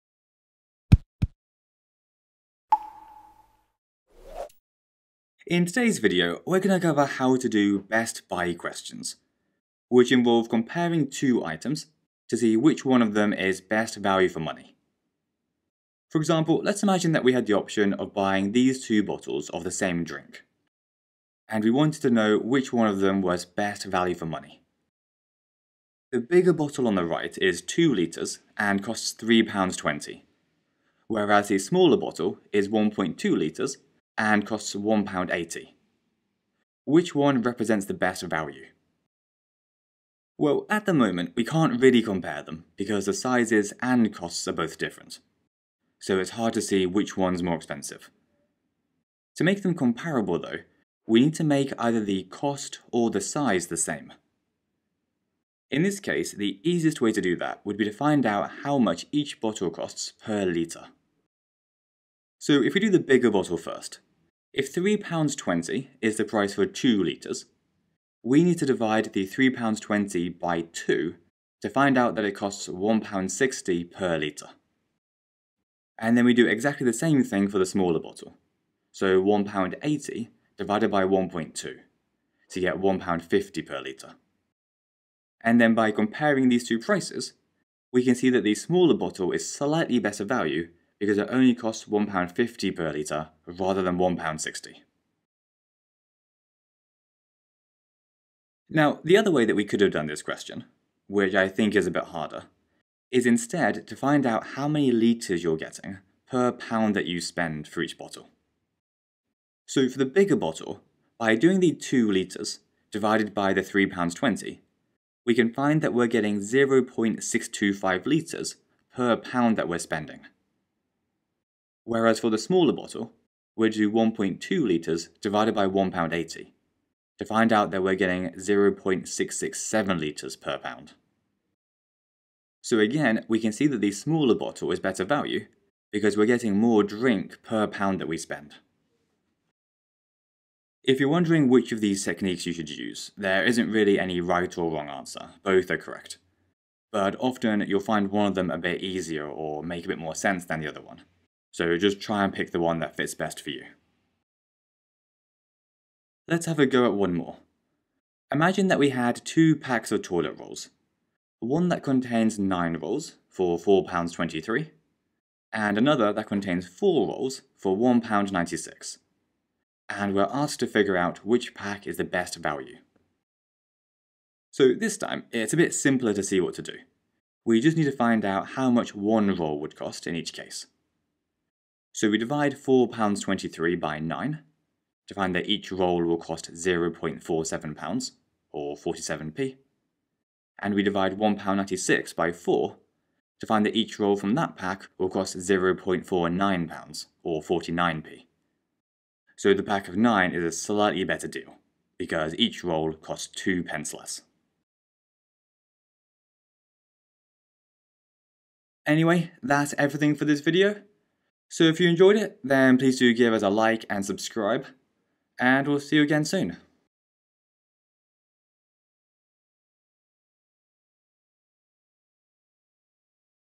GCSE Maths - How to Solve Best Buy Questions #88
This video covers:
- How to figure out which item is best value if we're given the prices and amounts of 2 options. E.g. if one bottle is 1.2L and costs £1.80, while another bottle is 2L and costs £3.20, which is better value?

A-level - Not on your course